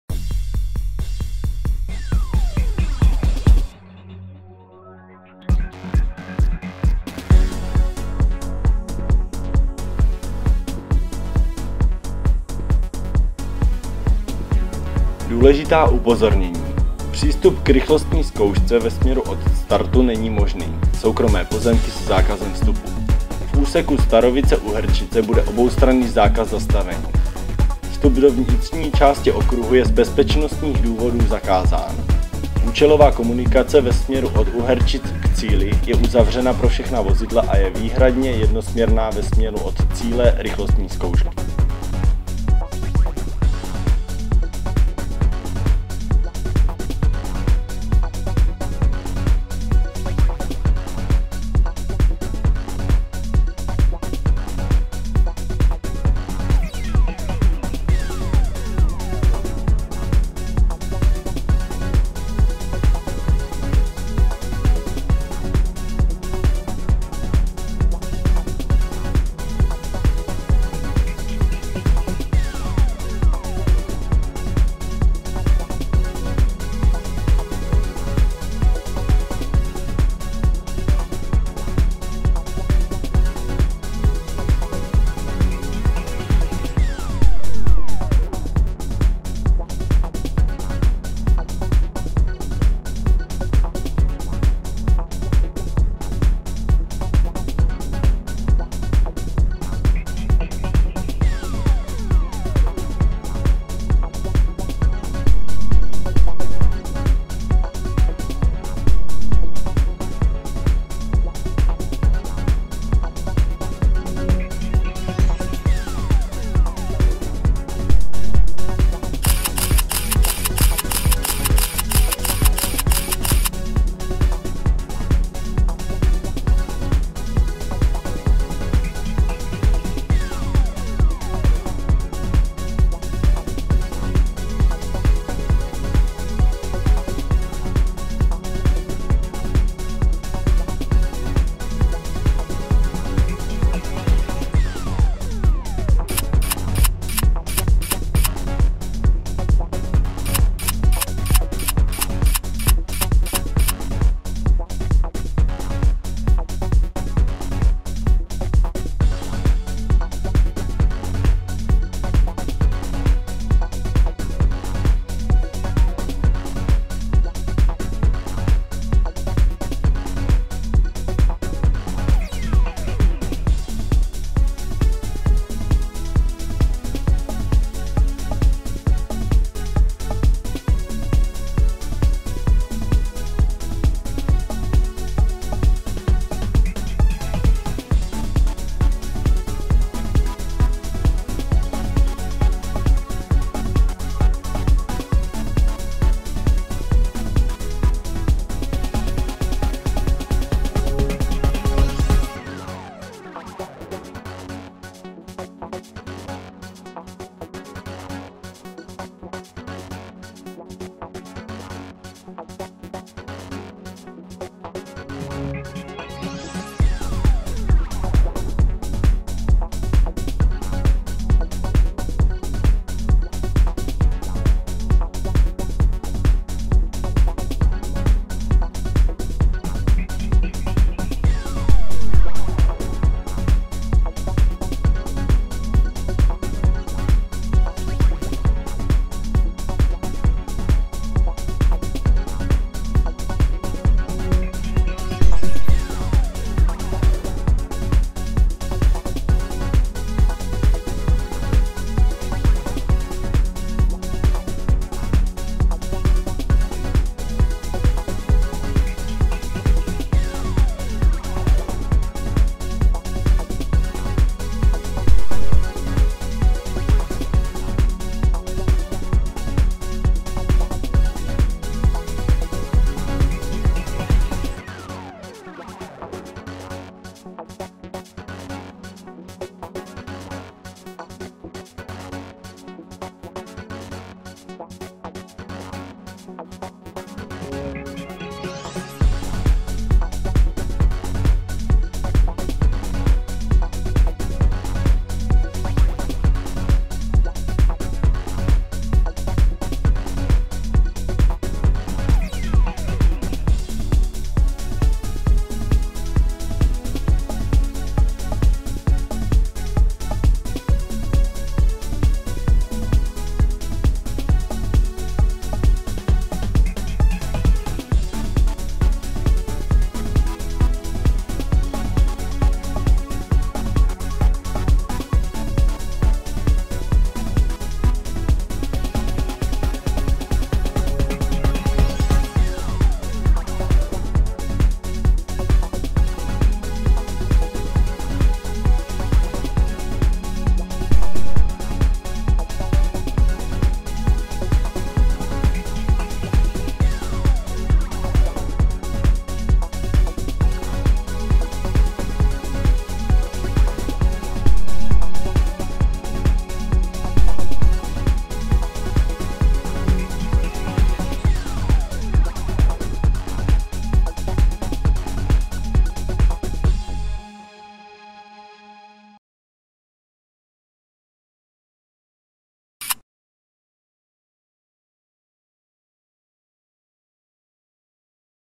Agrotec Petronas Syntium rally 2013: RZ 5, 7 Starovice, okruh -- 17,43 km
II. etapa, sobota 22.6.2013
Časy startů prvního vozidla: 8:24 hod. a 10:17 hod

Důležitá upozornění
1. Přístup k RZ ve směru od startu není možný (soukromé pozemky se zákazem vstupu). V úseku Starovice -- Uherčice bude oboustranný zákaz zastavení
2. Vstup do vnitřní části okruhu je z bezpečnostních důvodů zakázán.
3. Účelová komunikace ve směru od Uherčic k cíli je uzavřena pro všechna vozidla a je výhradně jednosměrná ve směru od cíle RZ.

DM G Starovice, u koníčků
Příjezd do Starovic ze silnice č.425.
Popis místa:
Příjezd v prudkém klesání po úzkém, uskákaném asfaltu, dobrzdění a odbočení vpravo na úzký asfalt.

DM H Žebrák -- komentovaná divácké zóna
Příjezd po obslužných komunikacích od silnice Hustopeče -- Mikulov (odbočit na konci Hustopečí vpravo k areálu a pokračovat ihned vlevo do kopce podál plotu).
Popis místa:
Rychlý příjezd po úzkém asfaltu, dobrzdění a ostré odbočení vpravo přes hranu. Následuje série několika rychlých zatáček ve stoupání. Délka přístupného místa je cca 800 m.
Proti hraně bude umístěna VIP tribuna.

DM I Nadlesí
Příjezd po obslužných komunikacích od obce Uherčice. Po odbočení u zemědělského objektu na první křižovatce vlevo.
Popis místa:
Příjezd po úzkém asfaltu, dvojitá zatáčka a po cca 250 m dělící retardér (dvakrát se projíždí s odbočením, jedenkrát přímo. V místě je organizován přechod přes trať.